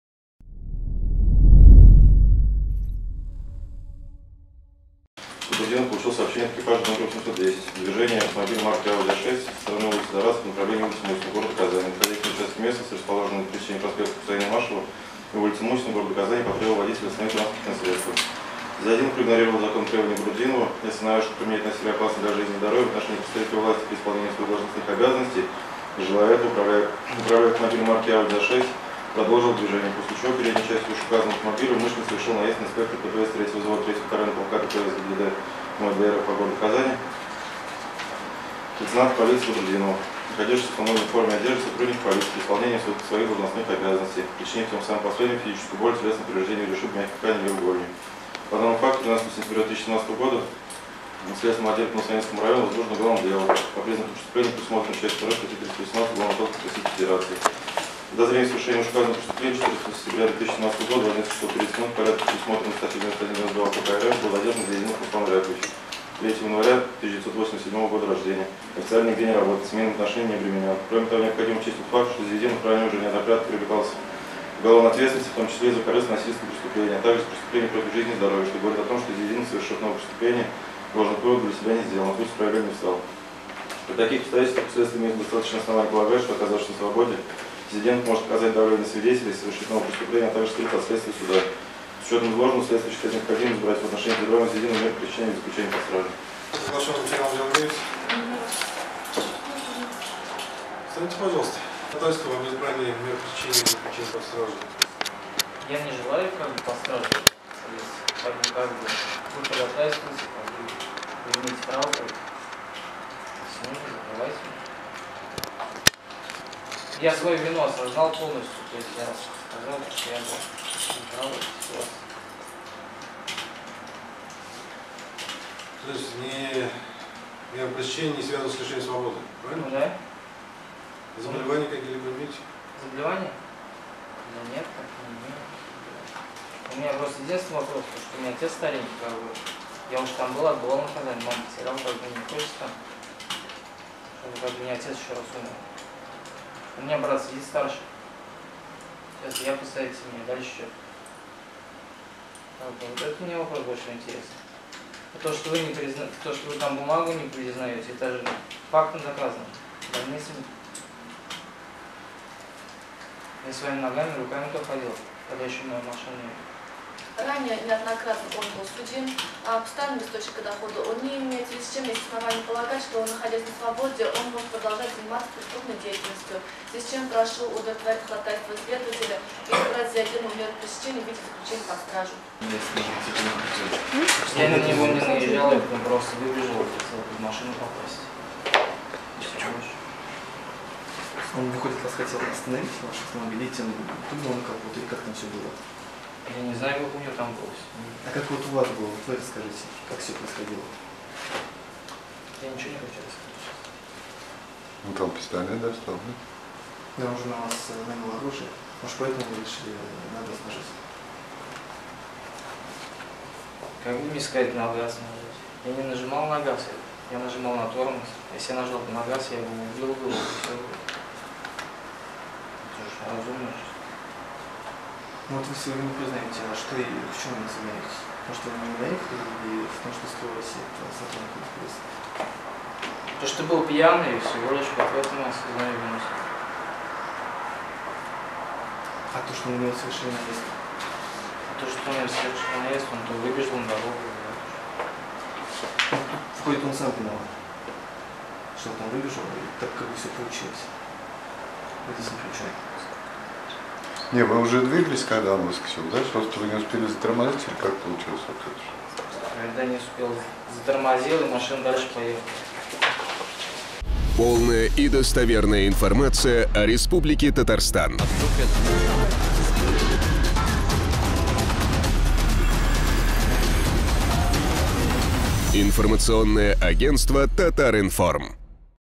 Ново-Савиновский суд арестовал наехавшего на инспектора ДПС водителя
Руслан Зиятдинов пробудет под стражей до 13 ноября.